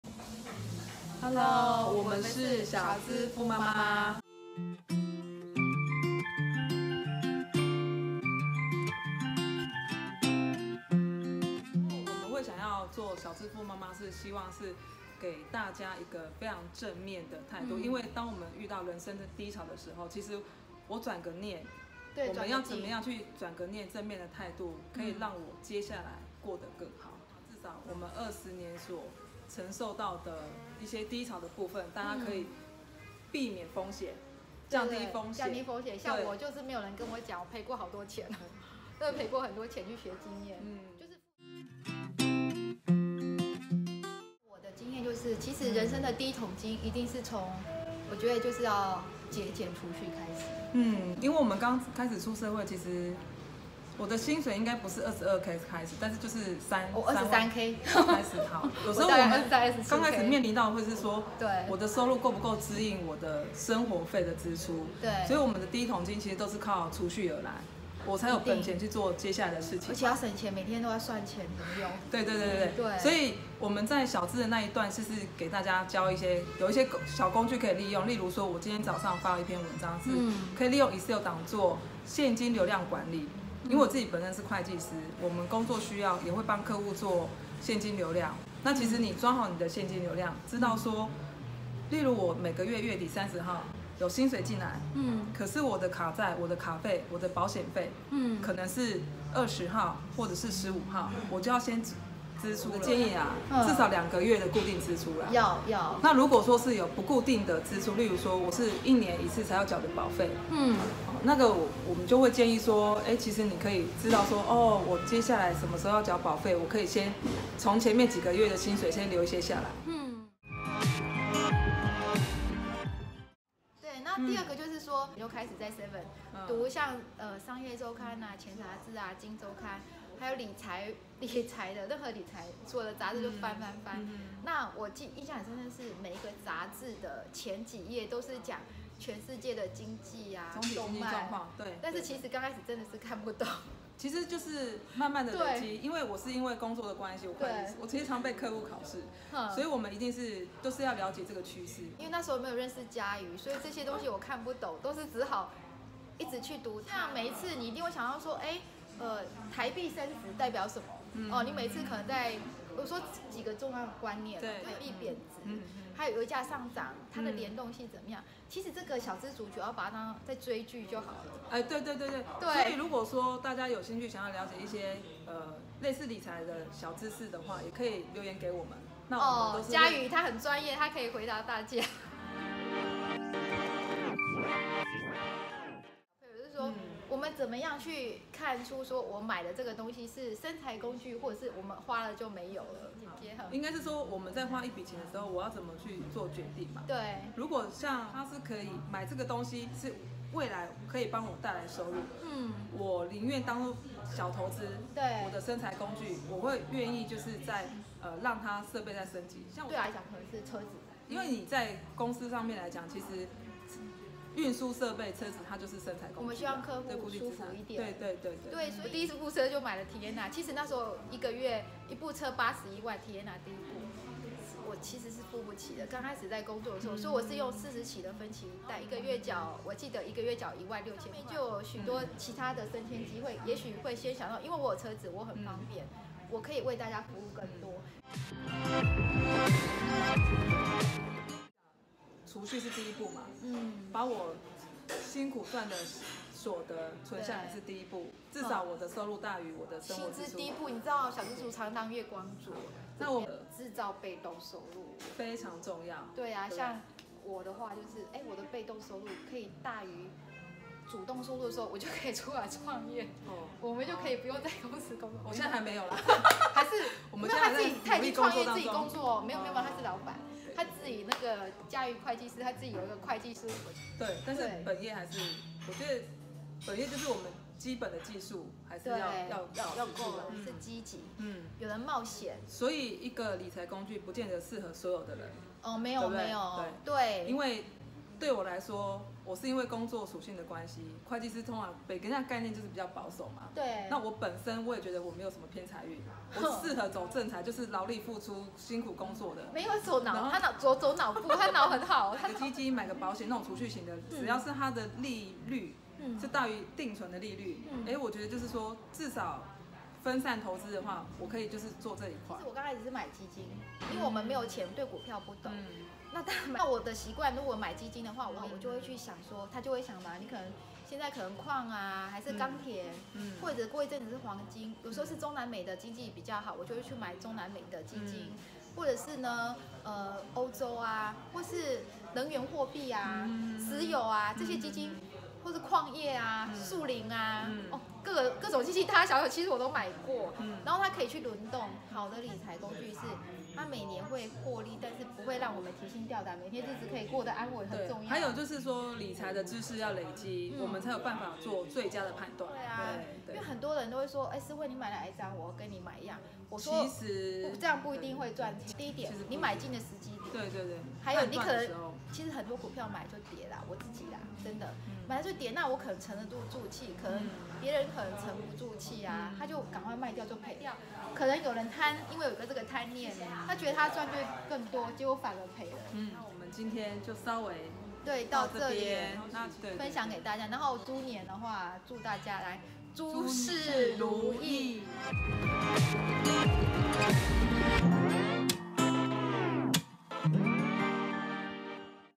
4個理財小撇步
分享佳瑜與詩慧從小資族到現在20年的理財經驗